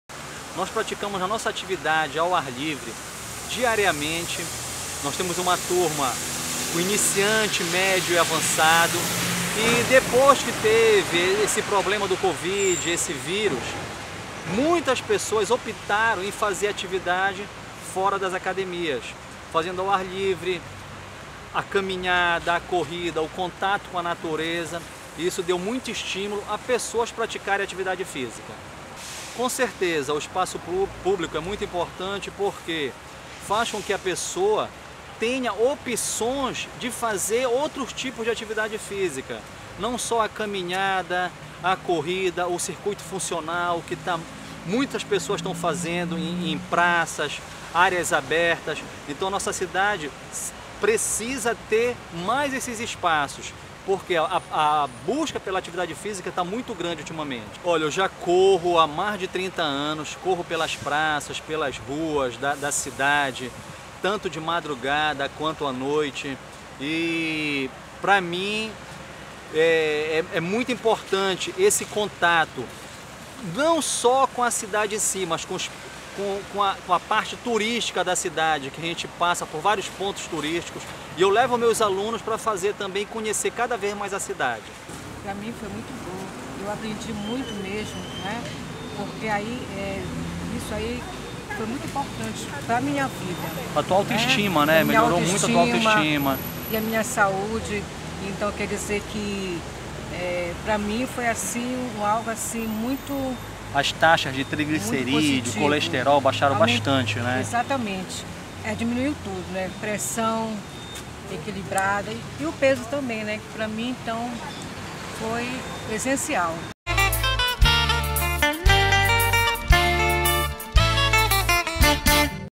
Conheça esportes típicos de Belém
Na capital paraense, observa-se o aumento de pessoas correndo, pedalando e se exercitando em espaços públicos.